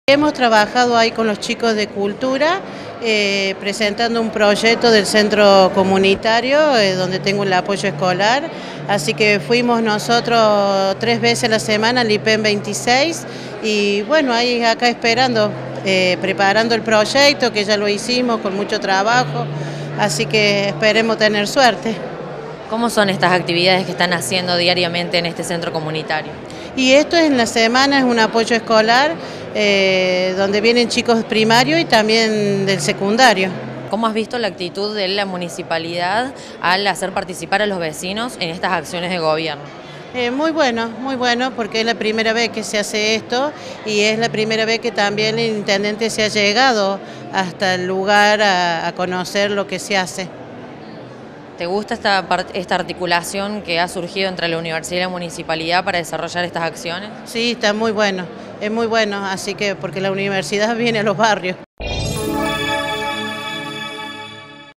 PRESUPUESTO PARTICIPATIVO
KARINA VEGA – VECINA DE RÍO CUARTO. VALORARON LA POSIBILIDAD DE HACER APORTES AL GOBIERNO MUNICIPAL.

Facebook.com/uniriotv
Twitter.com/uniriotv
Instagram.com/uniriotv
Web - uniriotv.unrc.edu.ar
o envianos un email a
UniRio TV
Universidad Nacional de Río Cuarto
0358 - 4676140